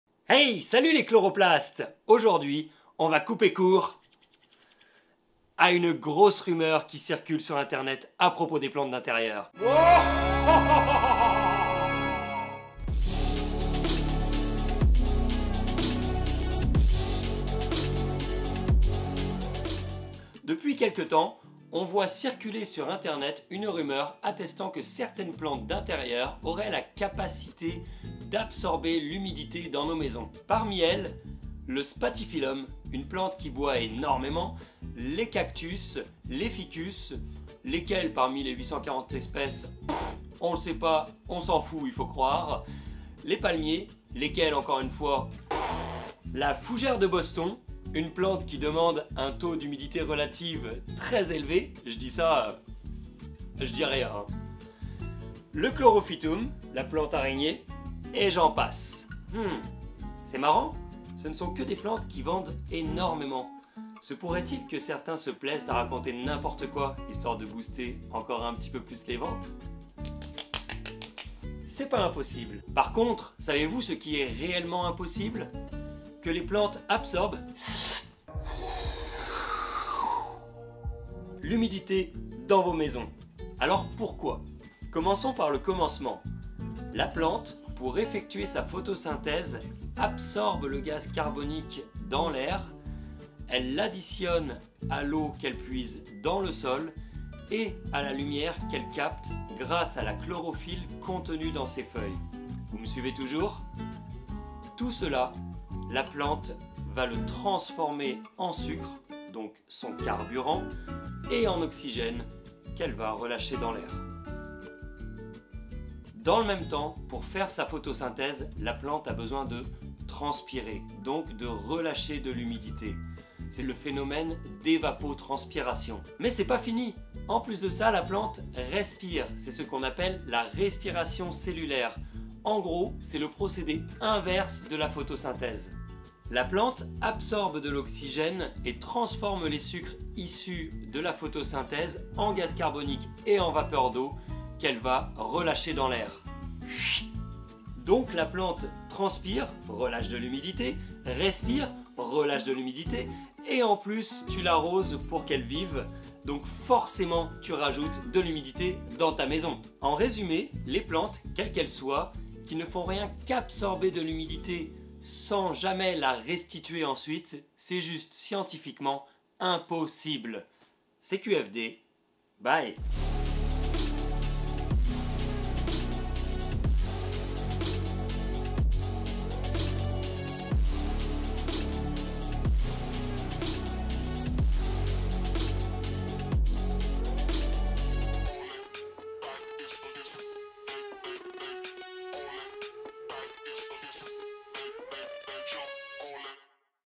Plantes déshumidifiantes : VRAI ou FAUX?
On entend souvent parler de plantes qui absorberaient l'humidité dans nos intérieurs.
Mais est-ce vraiment possible?
Pour y répondre, un peu de science végétale s'impose.
Et quelque chose me dit que la photosynthèse et la respiration cellulaire de la plante détiennent sans doute la réponse!
🎬Bon visionnage!

*Chloroplaste : composant des cellules végétales. C'est la partie cellulaire contenant la chlorophylle et assurant la photosynthèse.

0:00 Introduction
0:28 La Rumeur
1:32 La Photosynthèse
2:03 L'évapotranspiration
2:14 La respiration cellulaire
2:35 CQFD

Aussi court et bref qu'il soit, cette vidéo contient un extrait de la Méthode Wim Hof.
Rappel important à ce sujet : ne jamais pratiquer les exercices de respiration de la Méthode Wim Hof dans une piscine, avant d'aller sous l'eau, sous la douche ou de piloter un véhicule ou dans toute autre circonstance où la perte de conscience pourrait entraîner des dommages corporels. Pratiquez toujours en position assise ou allongée dans un environnement sûr.
Pour plus d'information et si le sujet vous intéresse, n'hésitez pas à aller visiter le
🌴 Les plantes ont besoin de respirer, nous aussi! 😄

Pour apprendre plein de petits trucs et astuces sur les plantes, retrouve-moi entre 2 coups de pelle et de sécateur :